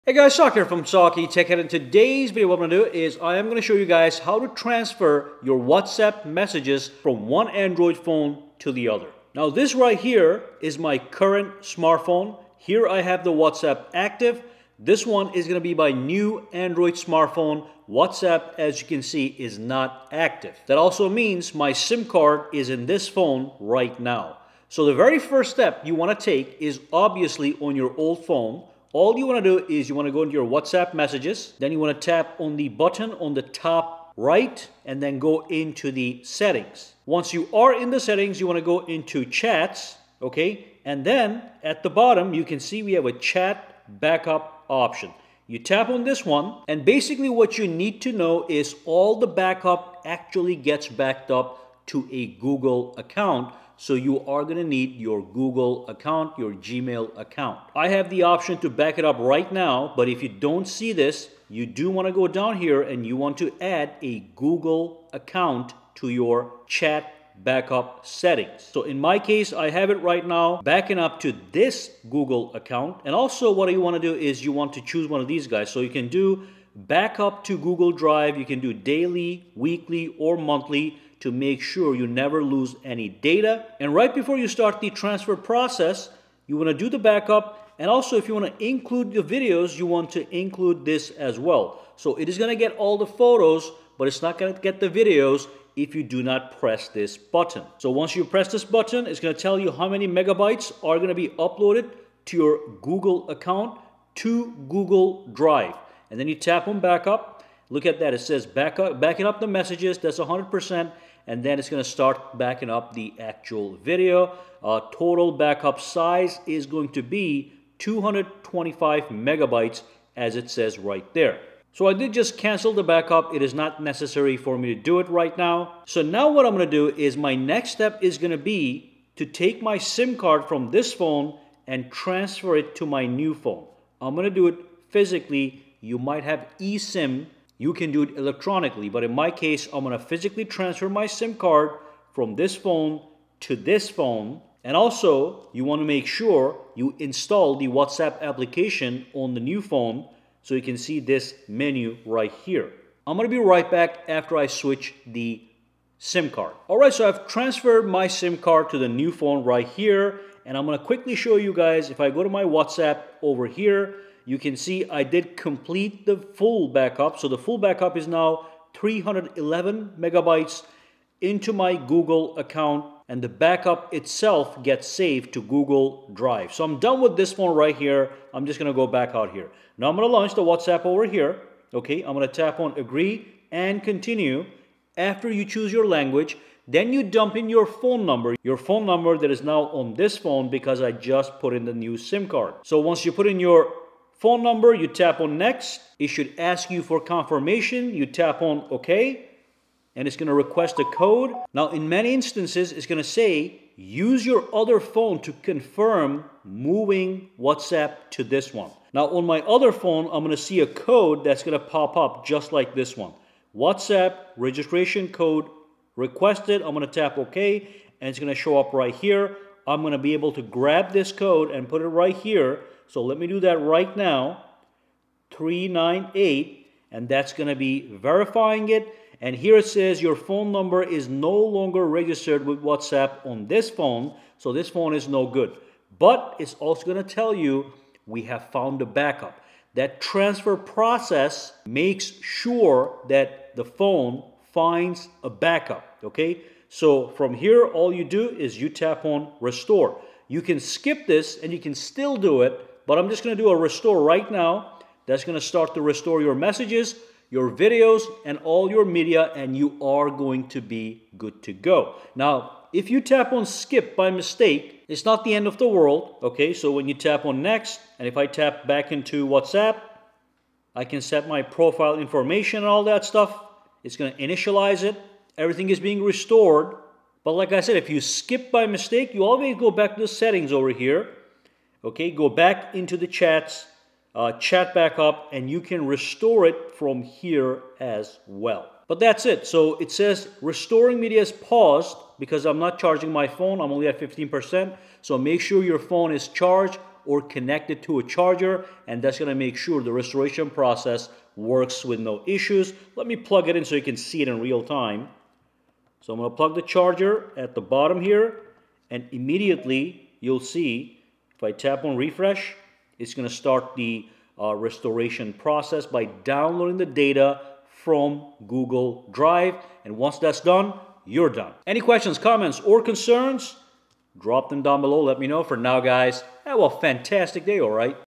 How To Transfer WhatsApp Messages from Old Android to New Android Phone (Free and Fast)
IN this video, I will show you crystal clear steps on how to transfer your WhatsApp messages from your old android smartphone to your new android smartphone.

As we transfer WhatsApp messages from old phone to new android phone, you will also be able to transfer all the photos and videos found inside your WhatsApp.